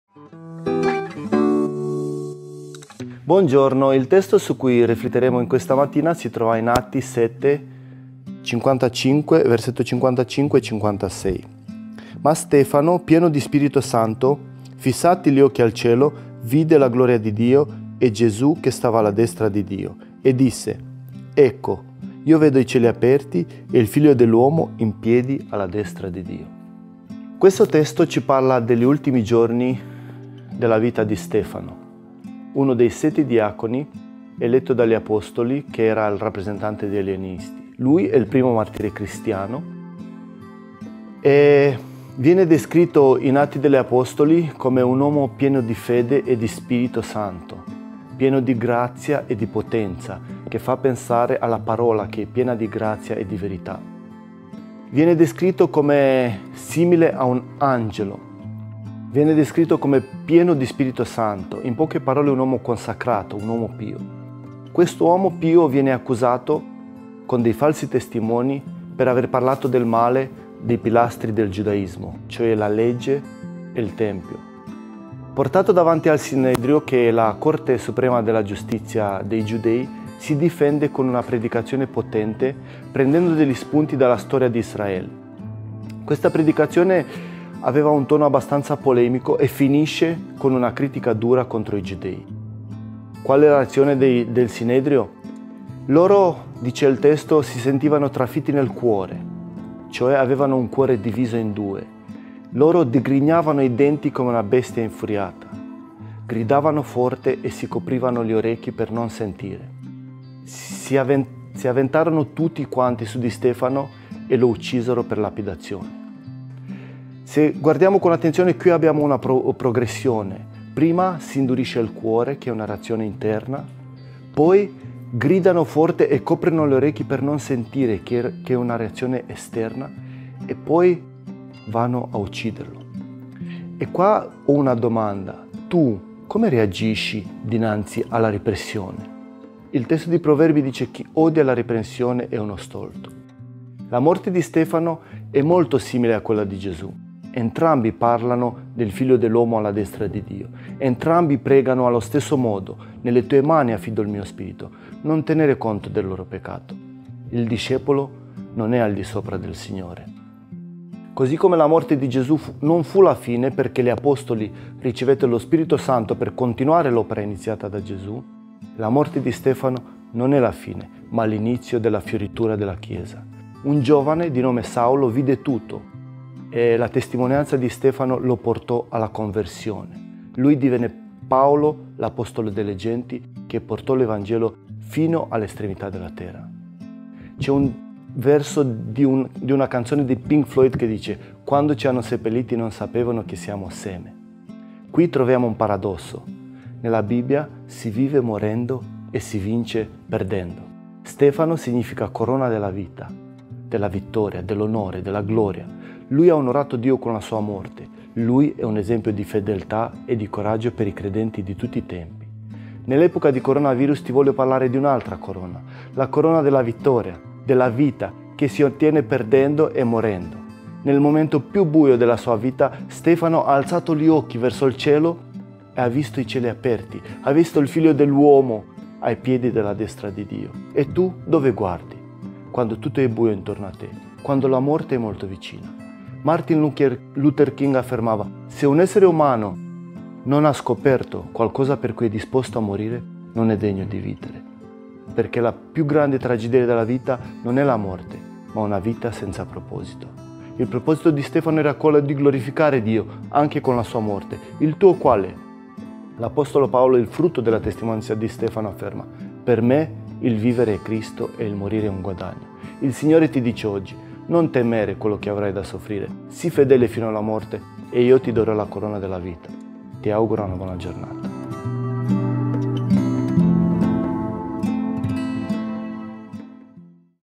Giovedi 13 maggio 2021
Atti 7:55,56 - Ma Stefano, pieno di Spirito Santo, fissati gli occhi al cielo, vide la gloria di Dio e Gesù che stava alla sua destra, e disse: «Ecco, io vedo i cieli aperti, e il Figlio dell&#39;uomo in piedi alla destra di Dio»

Meditazione giornaliera Apri la porta del tuo cuore. In studio Alexandru Chiuzbaian